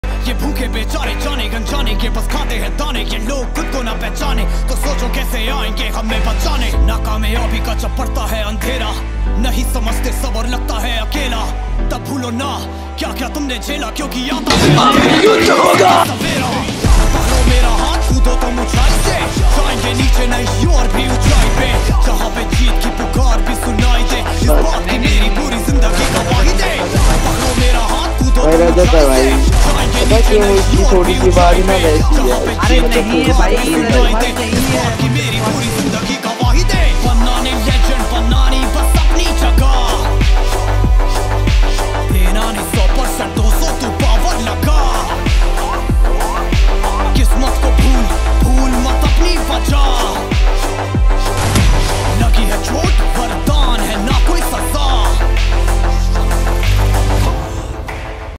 Kaal yt expose revenge
Get ready for an action gaming experience like never before. Id ka Name kar diya change Kaat Yt se kiya jai shree ram🔥Aur 2 vs Squad ki hui match against Ravan Team.

 • GUILD TEST PRANK ON CUTE V BADGE GIRL...

 • GUILD KICK PRANK WITH BIG YOUTUBER 😱🔥...

 • TOP 1 GLOBAL PLAYER VS 3 RED CREMINAL...